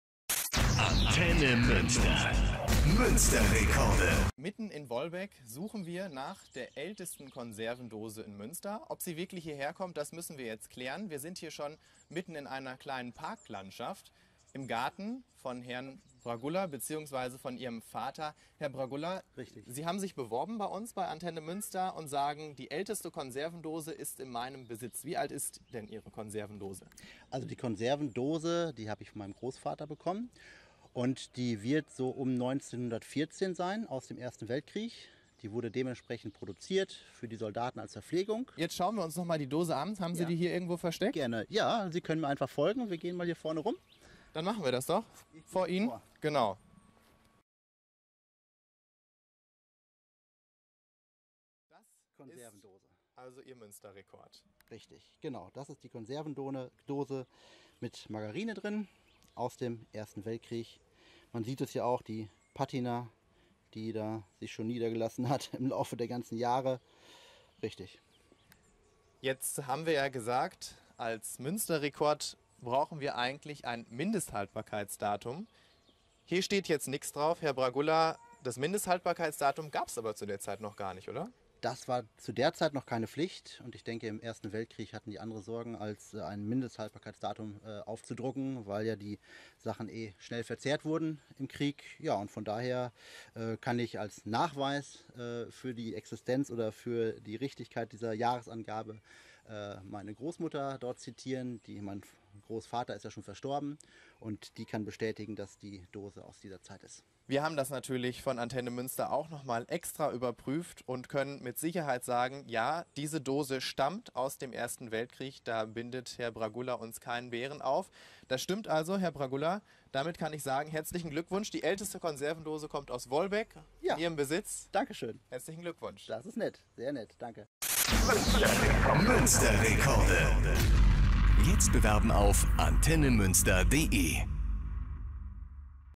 Münster-Rekorde: Die Rekord-Dose
Die älteste Konserverndose aus Münster kommt aus dem 1. Weltkrieg. Münster Rekorde-Reporter Tim Diekmann hat den Gewinner besucht und den Check gemacht: Ist die Dose wirklich original?